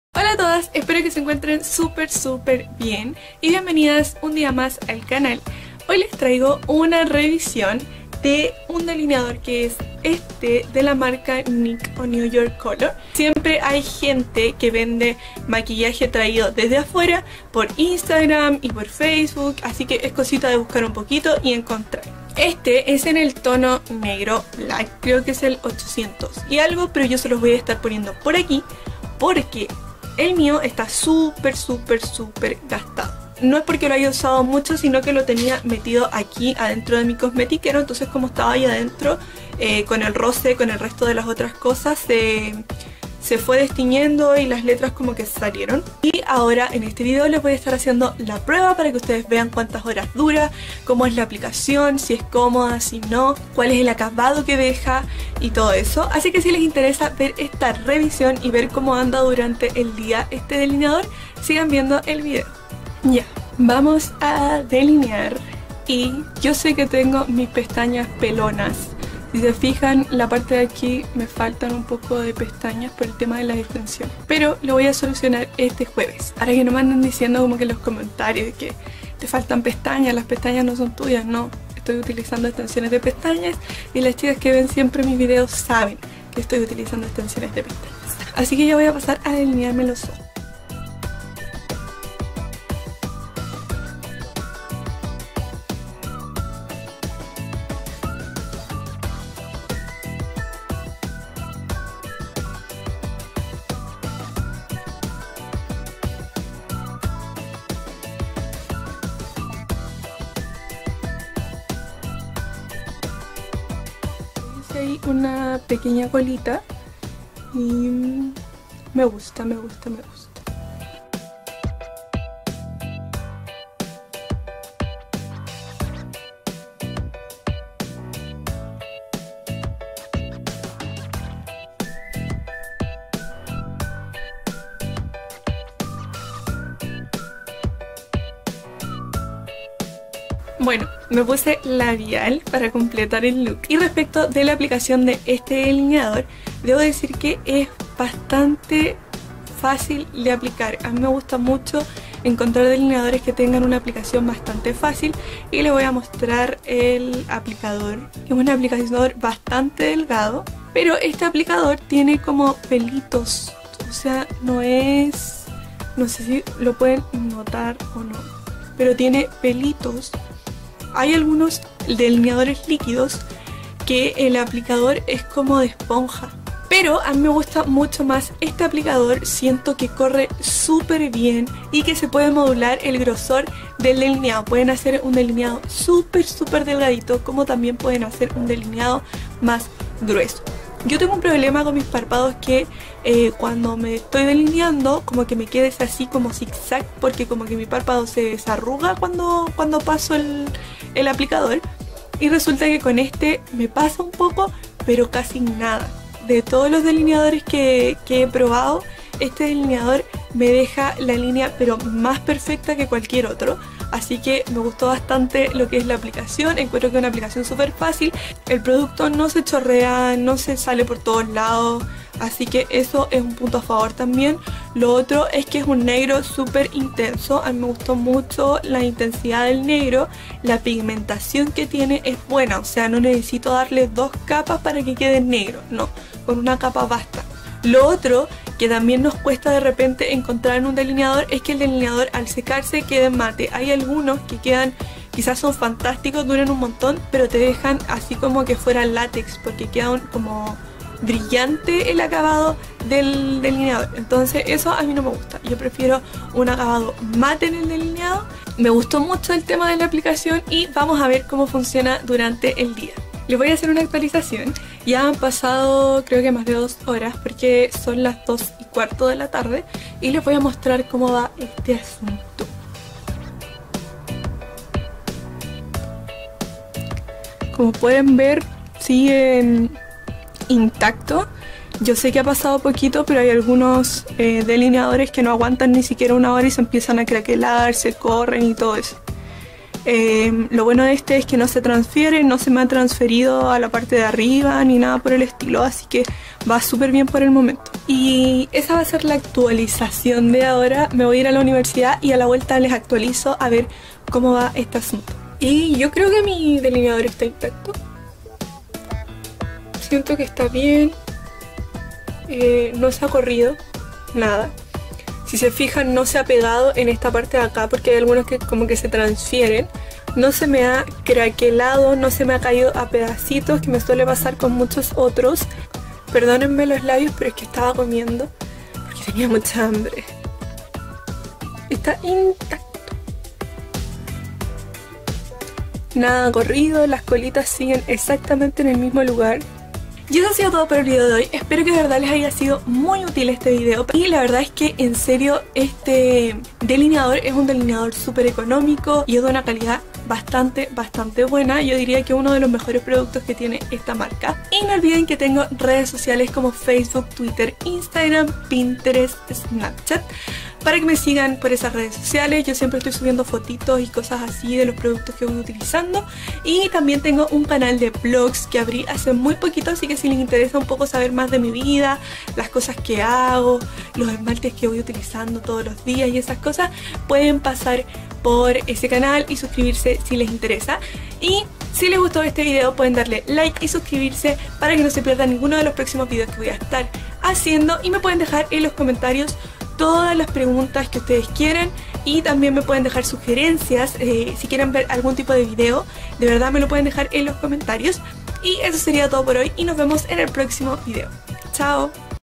Prueba y revisión! Liquid Eyeliner NYC
Hola a todas hoy les traigo esta revisión de un producto que me tiene muy contenta, amo encontrar cosas buenas y económicas, se trata del delineador liquido de la marca NYC, me lo trajo mi mejor amiga de Estados Unidos, se que venden esa marca aquí en Chile también así que pueden encontrarla, es económico y ademas de buena calidad, de verdad me ha sorprendido un montón, en el video solo muestro una prueba de 8 horas app, pero olvide quitármelo y al otro día seguía intacto! es maravilloso, no es a prueba de agua por lo que es fácil de retirar pero no se mueve en todo el día, aunque quizás para alguien con parpados muy grasos no funcione bien, pero de todas formas espero les sea útil!